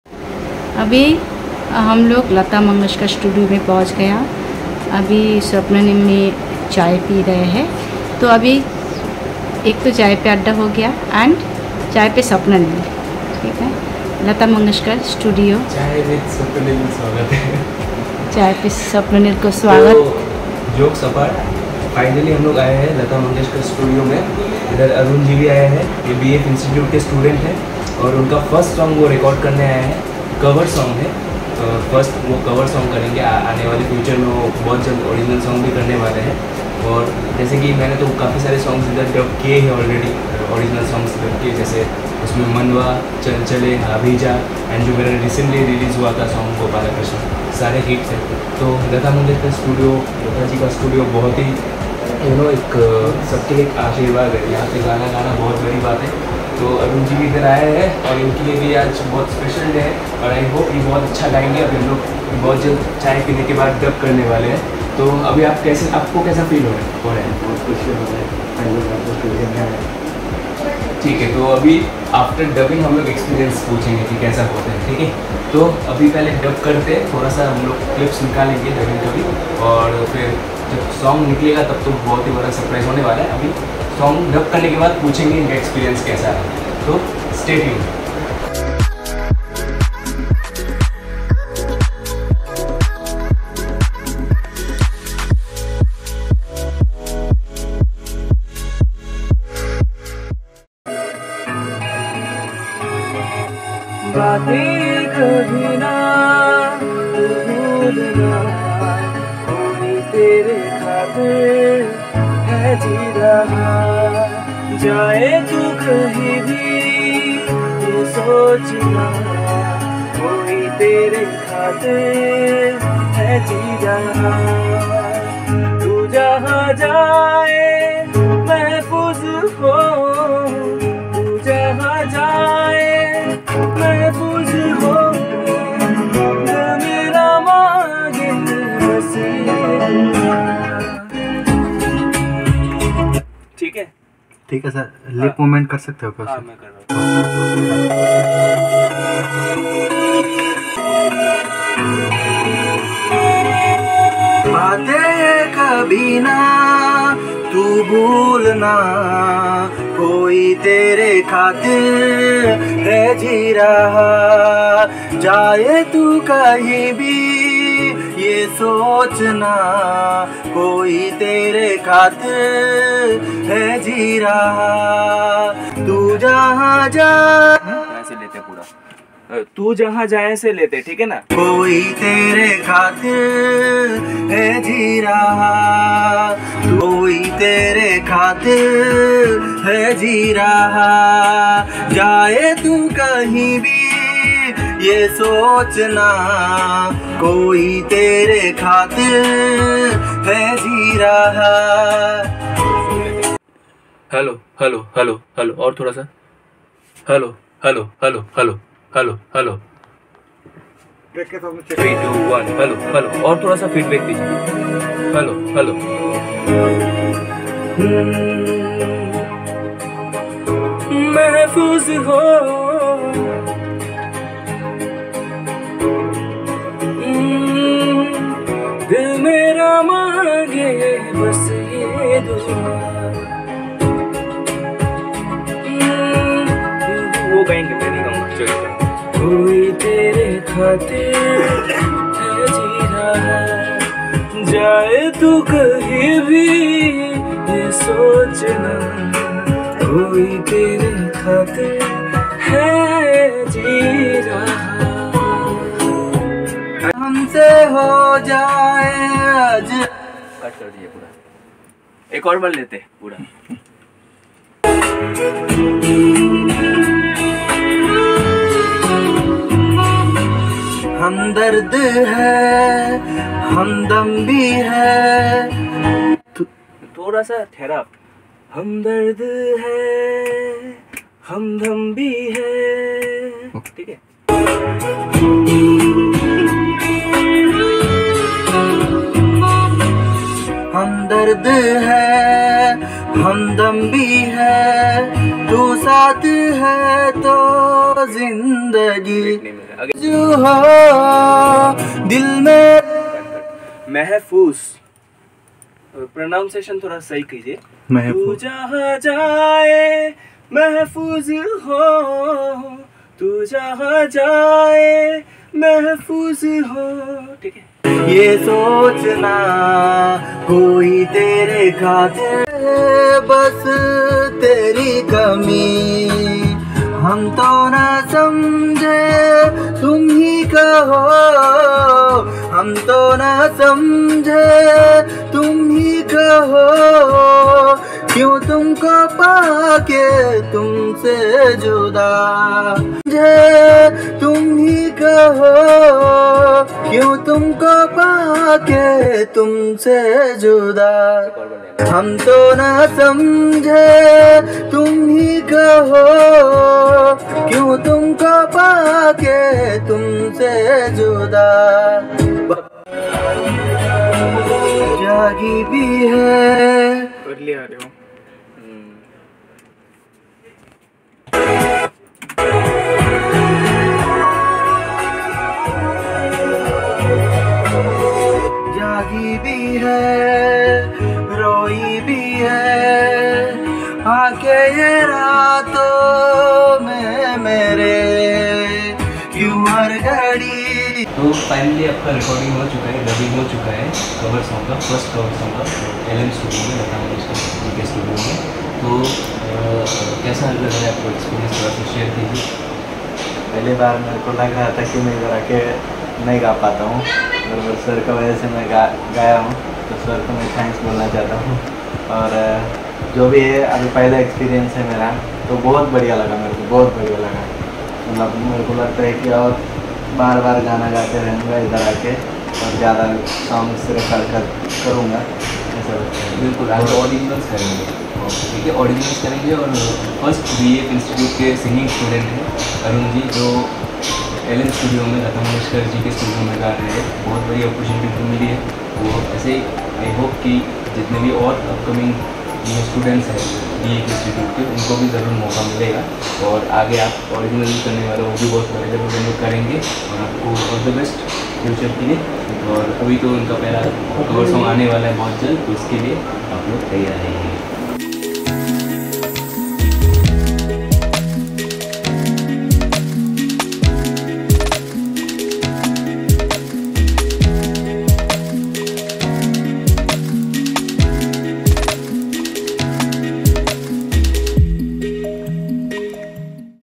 BEF Institute Student First Time Dubbing/Recording at Lata Mangeshkar Studio | Swapnanil Bhadra Sir
BEF Institute Student "Arun Kumar" First Time Dubbing/Recording at Lata Mangeshkar Studio Mumbai Under Guidance of Swapnanil Bhadra Sir.